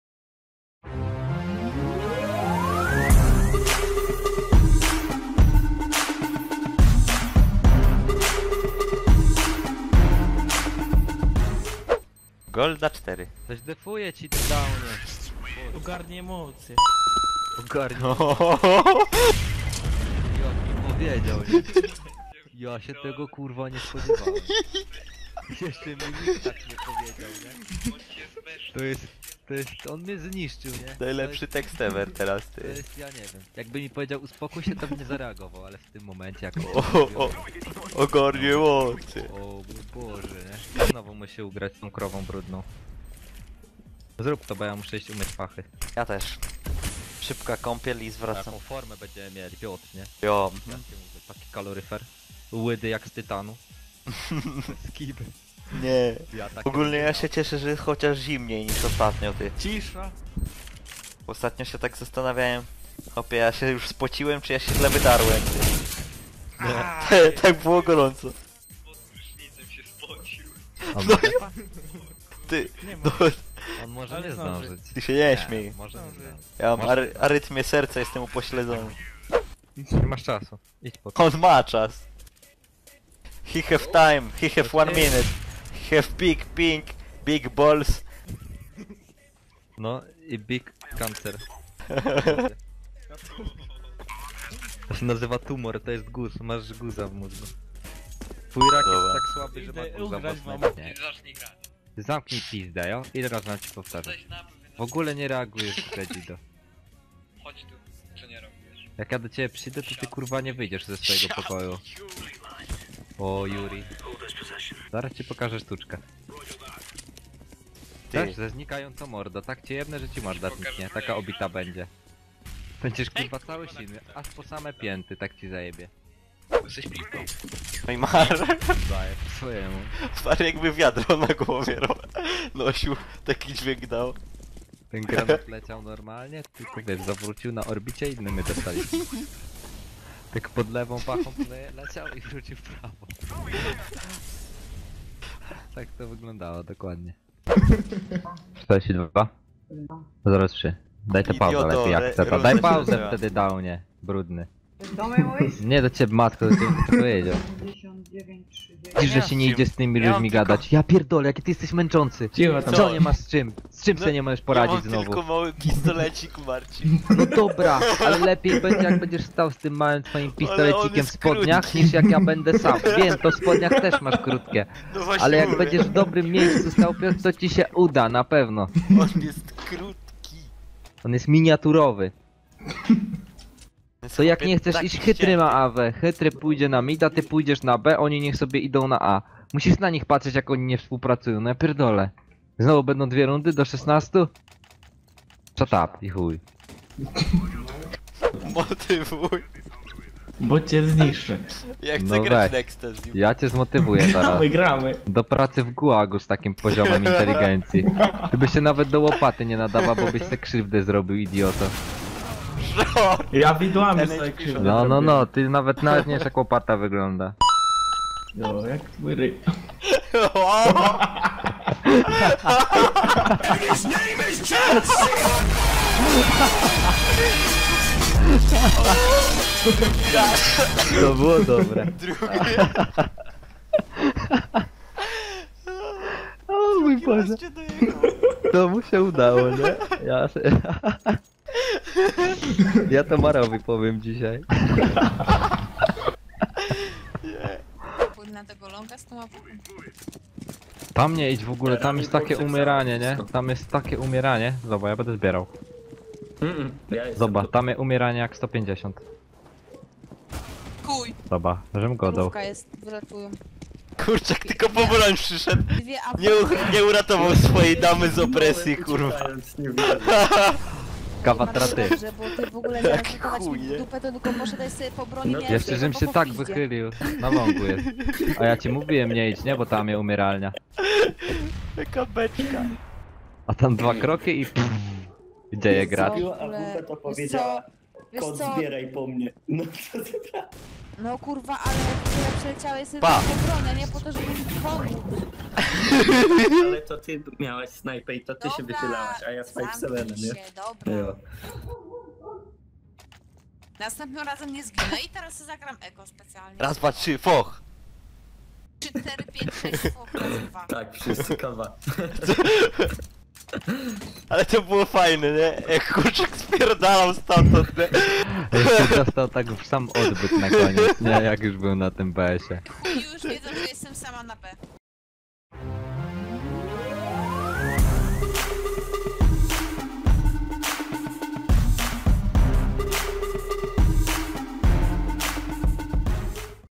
FUNNY MOMENTS - KawjorekGaming #5
Poleciał, kolejny filmik z kolejmymi chorymi tekstami i akcjami z naszego streama, w tej części zobaczycie między innymi: uciekające kurczaki, SplaciK vs. Mr.Chytry cebul battle, krótkie pistolety, zagłębimy się trochę w kosmos i orbity no i oczywiście spocimy się pod prysznicem!
Chcesz więcej chorych akcji? Zajrzyj tutaj:
Streamujemy każdego dnia w serwisie Twitch! Tego nie możesz przegapić!
P.S: Pozdrawiam wszystkich, którzy przyczynili się do powstania tego chorego filmu.